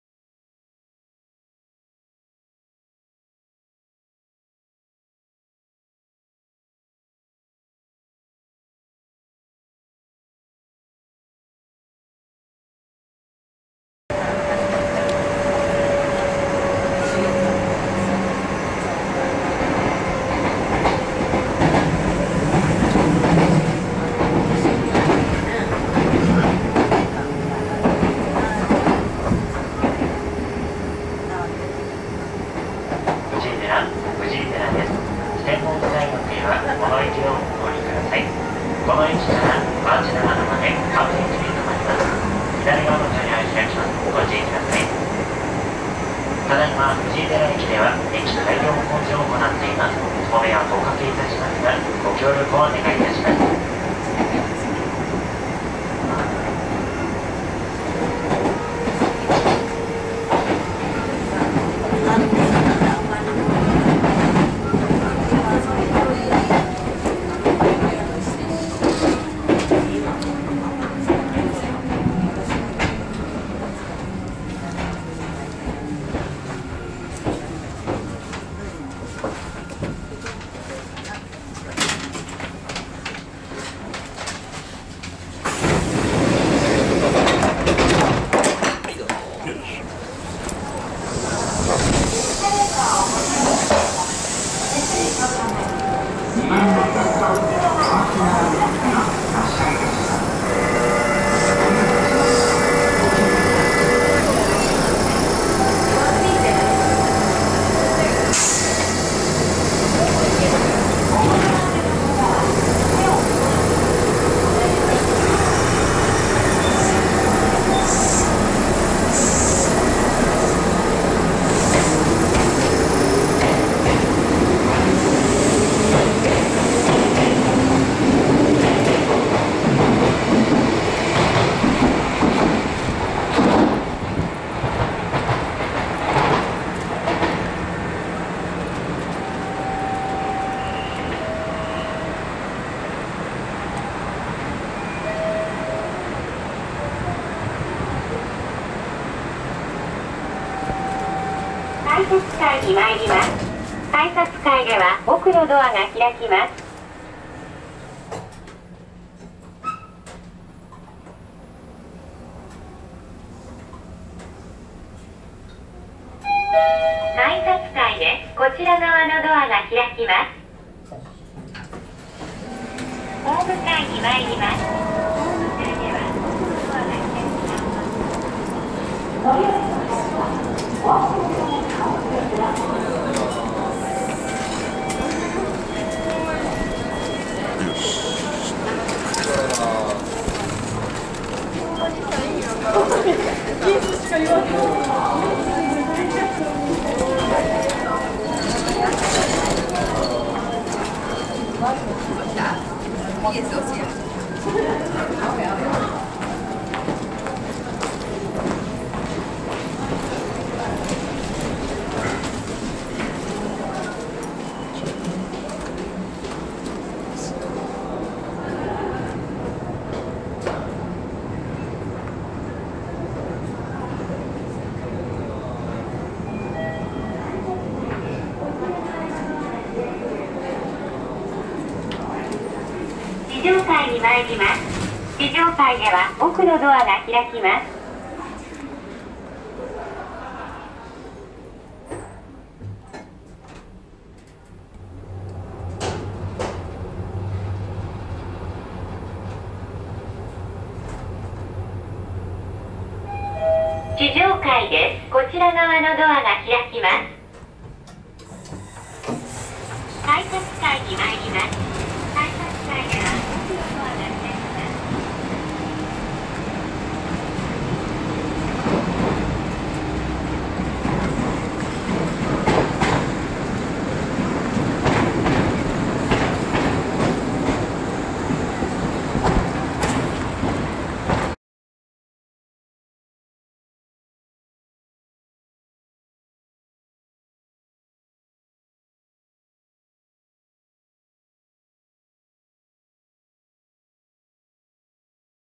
車いす道中記 近鉄南大阪線藤井寺駅下車 大阪府藤井寺市編
車いすで、近鉄南大阪線の藤井寺駅で下車した動画です。